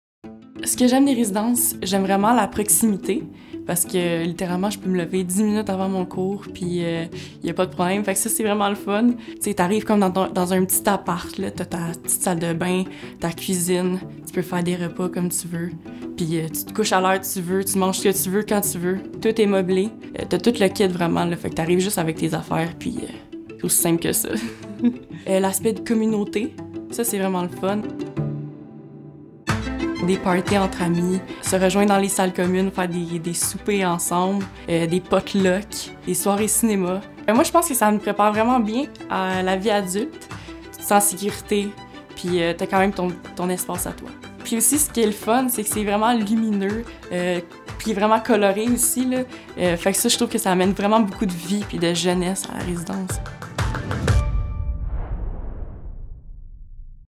La résidence du Cégep de Valleyfield - Témoignage de Juliane, étudiante et résidente
Vidéo tournée à l'hiver 2024